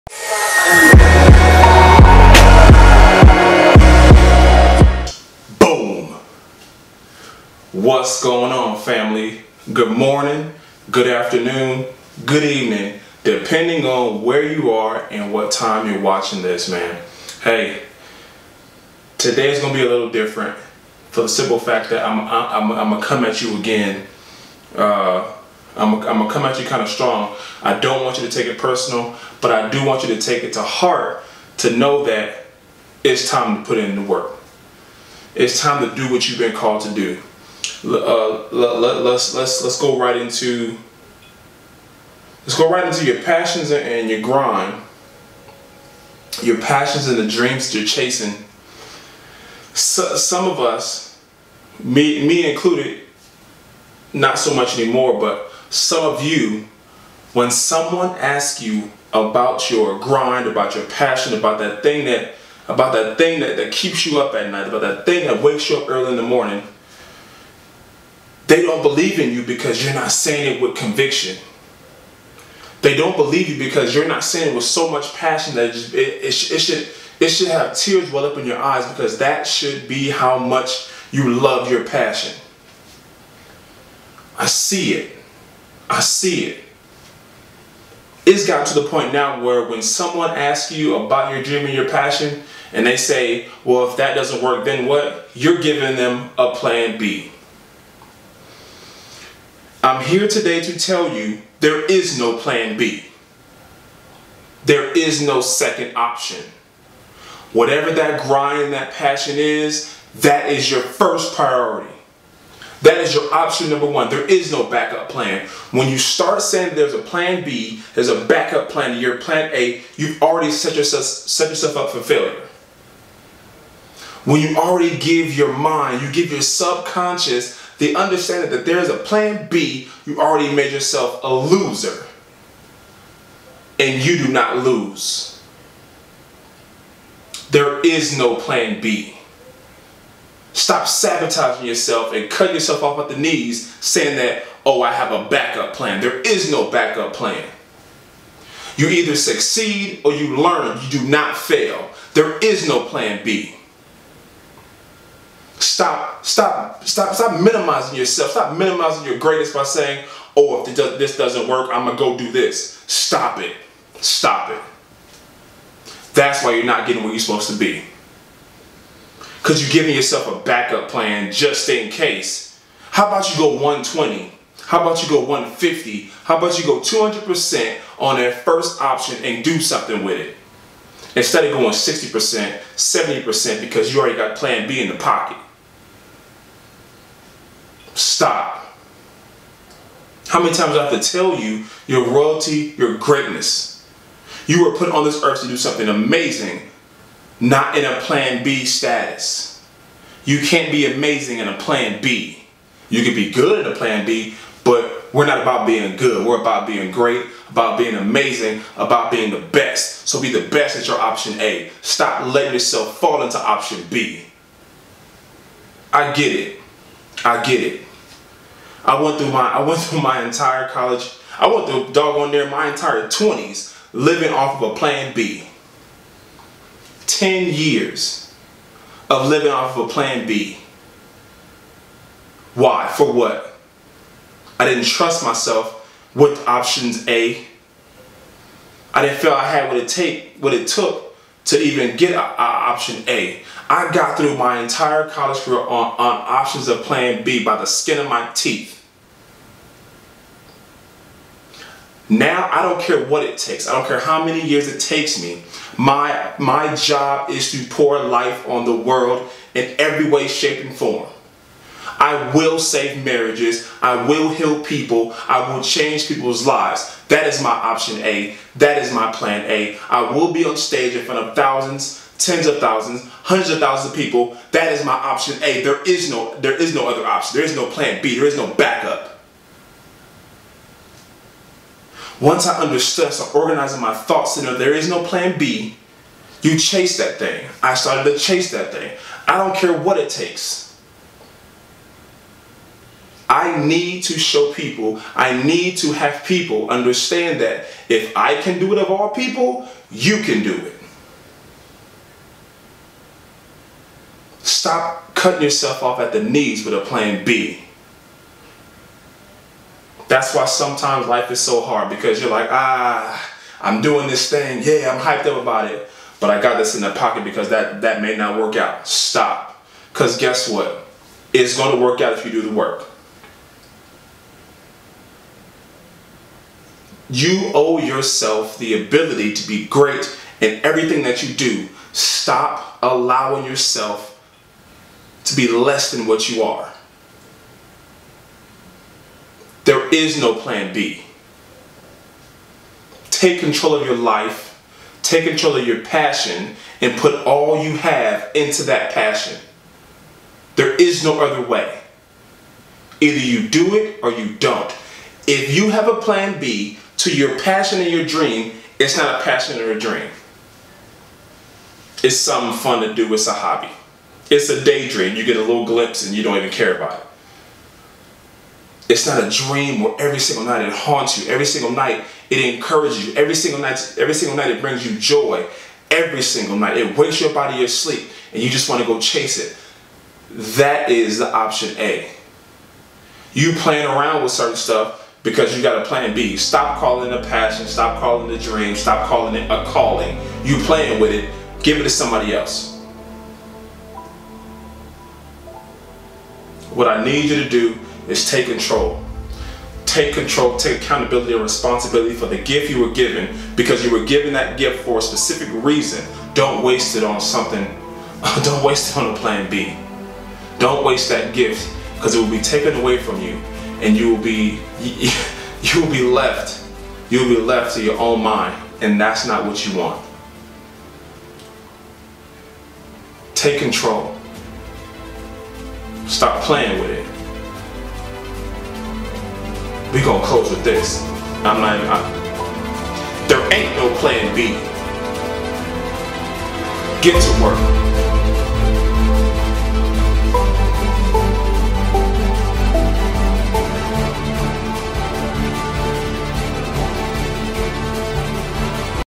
PLAN B | DONT TAKE IT | PASSION
Plan B is not an option in the life of a dedicated person.  We need to stop saying I have a plan B and start to Dive into the seriousness of our plan A! Fight for your life to accomplish your goals!